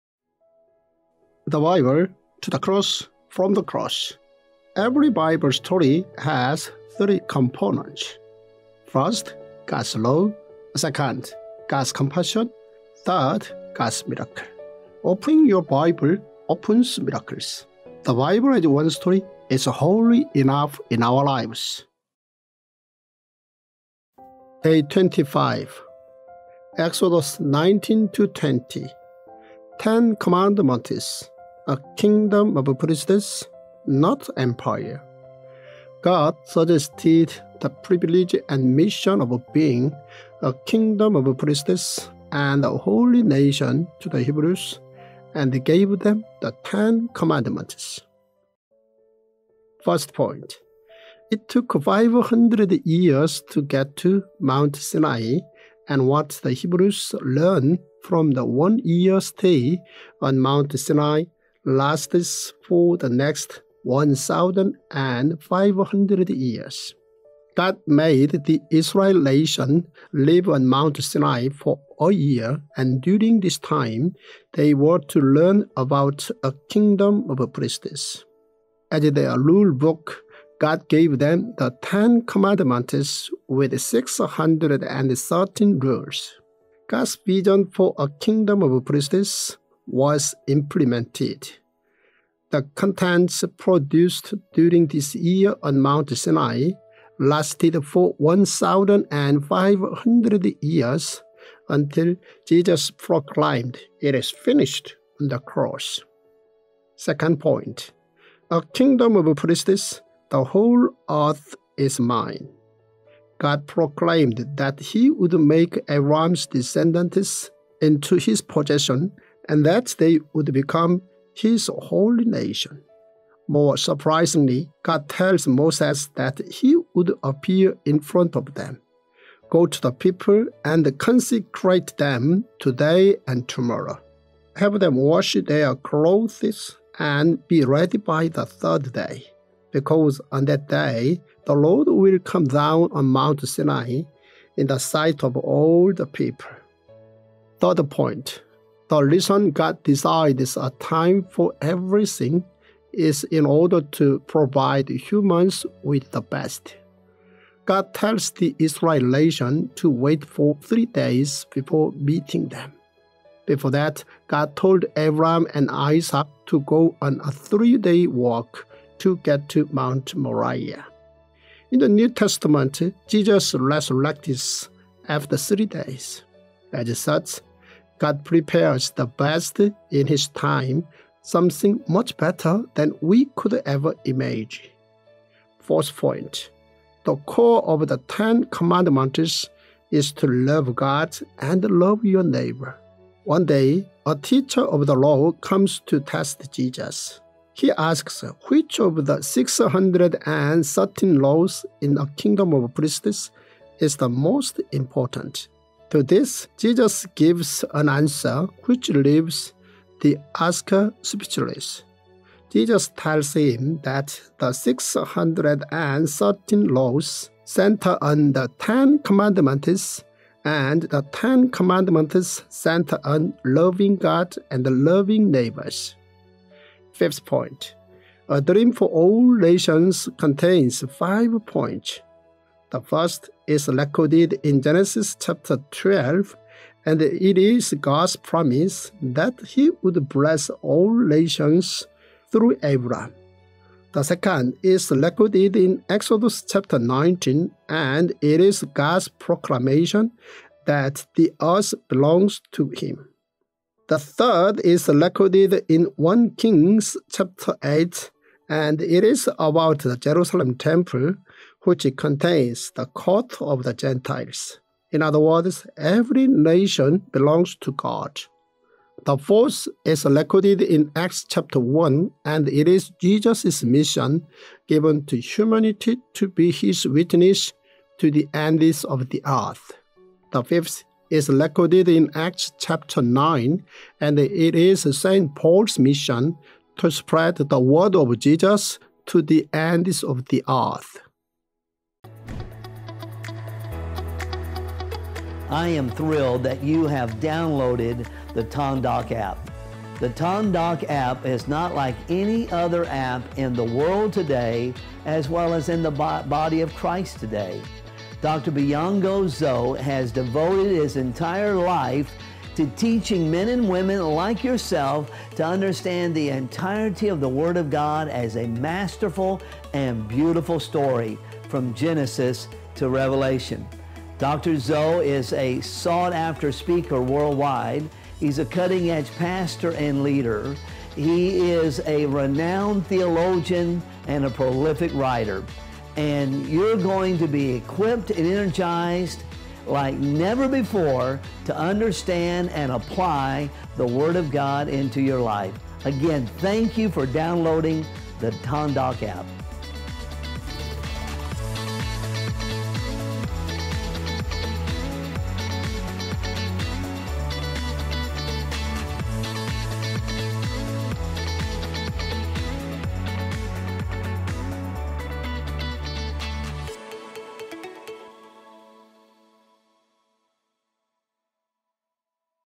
Tongdok Bible | Day 25 Exodus 19-20 | Ten Commandments: A Kingdom of Priests, not Empire
Tongdok Bible | Day 25 (Exodus 19-20)

Ten Commandments: A Kingdom of Priests, not Empire
God suggested the privilege and mission of being ‘A kingdom of Priests and a holy nation’ to the Hebrews and gave them the Ten Commandments.